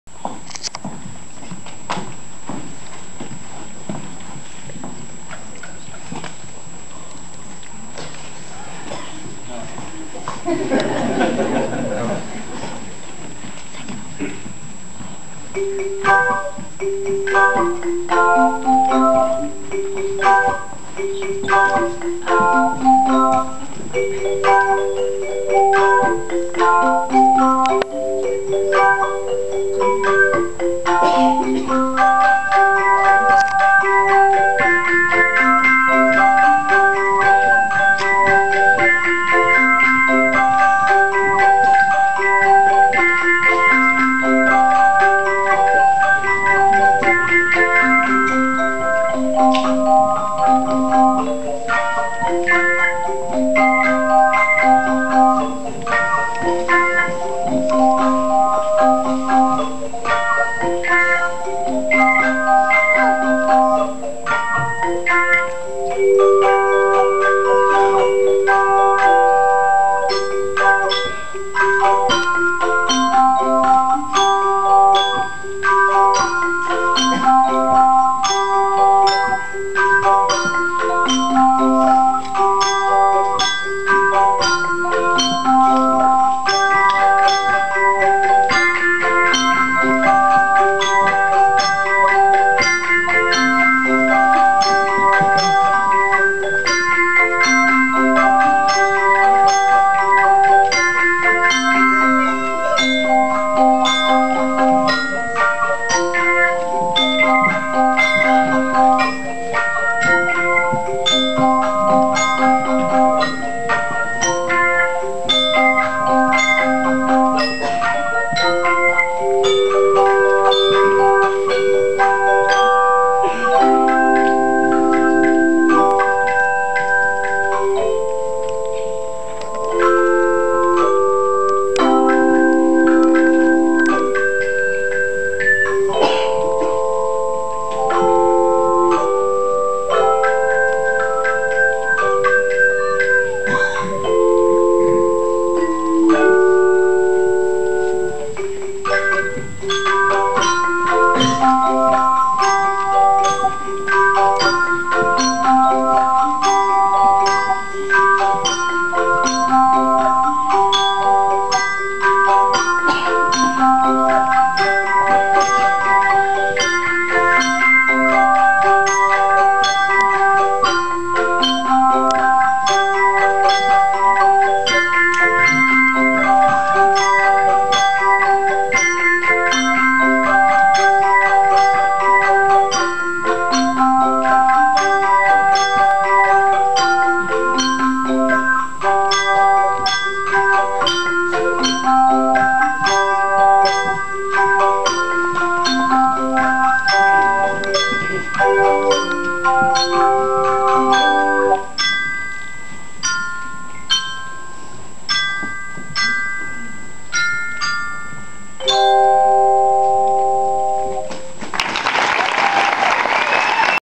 Pembroke Academy Christmas Show 2009
Stephen playing the bells. What a handsome devil!